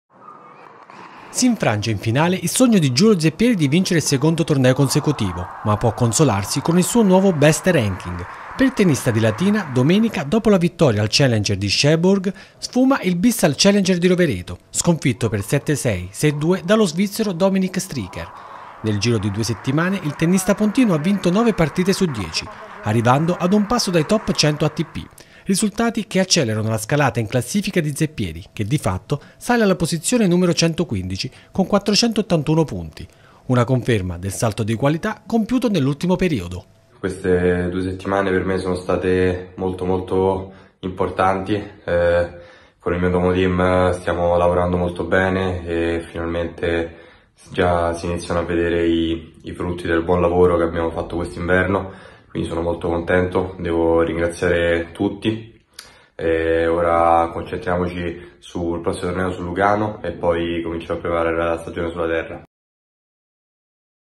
LATINA: Il tennista Giulio Zeppieri del 27-02-2025
Vittoria a Cherbourg, finale a Rovereto: il tennista di Latina, Giulio Zeppieri, inizia la settimana come numero 115 del ranking Atp. Ripercorriamo gli ultimi giorni.